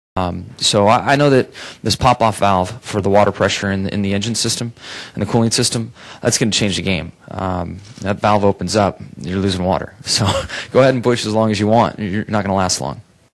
Jimmie Johnson on the NASCAR competition changes
Jimmie Johnson says the competition changes made by NASCAR will change how he races.